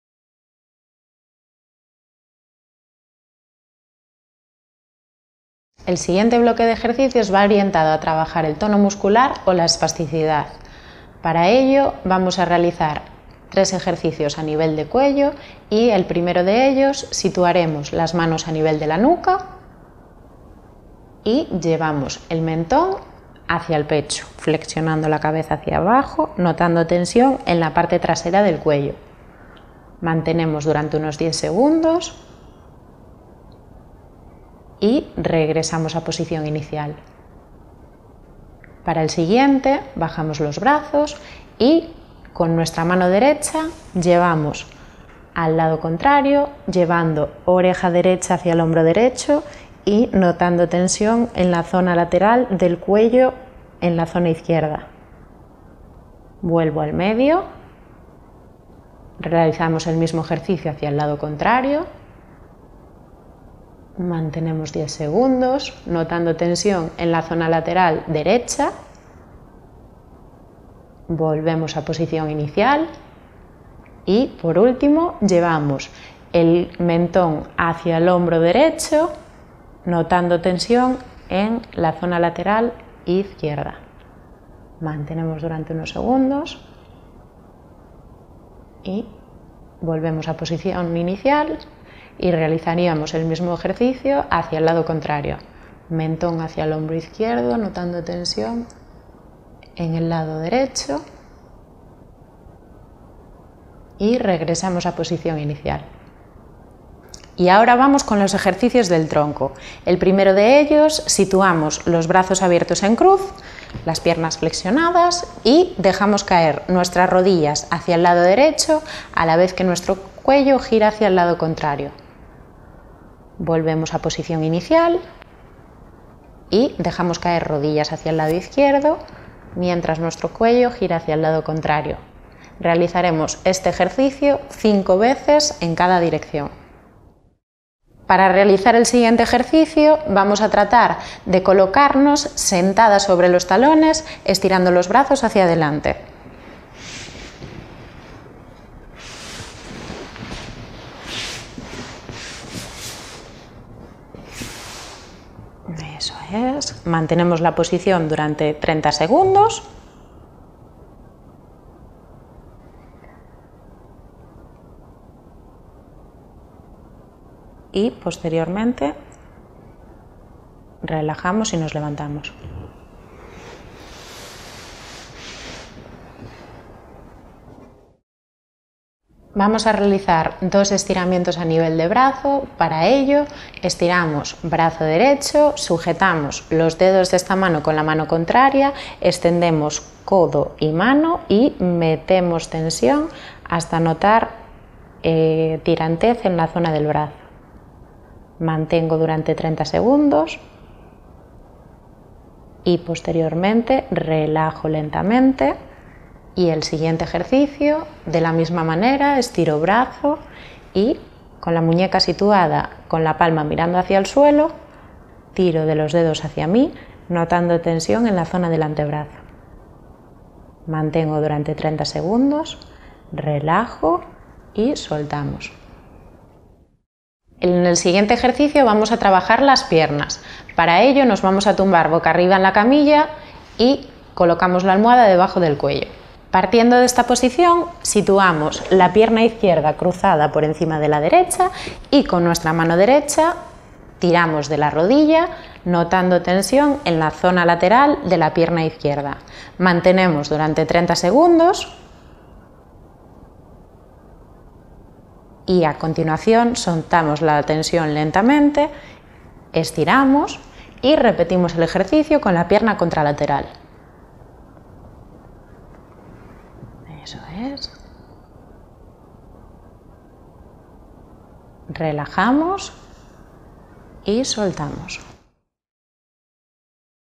Esclerosis Múltiple: Ejercicios de tono muscular
Los ejercicios de tono muscular alivian los síntomas de la espasticidad. Son estiramientos, que contribuyen a relajar los músculos.